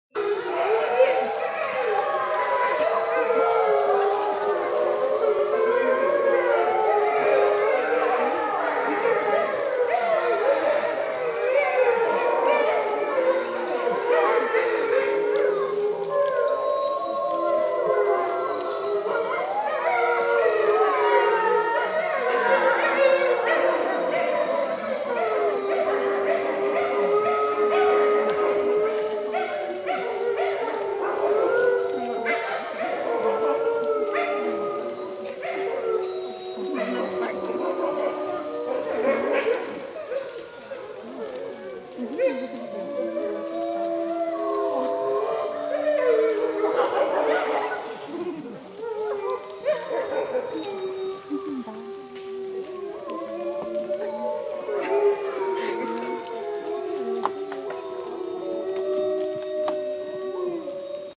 Wolves, Bays Mountain science camp trip
This year's trip for Pikeville Science and Math Camp was to Bays Mountain Park in  Tennessee.  Among the attractions is a pack of wolves who howled for us.  I wasn't able to get a very good view but I think you will enjoy hearing their "serenade".

For more about the camp go to: Pikeville Science and Math Camp 2012

P1200642.mov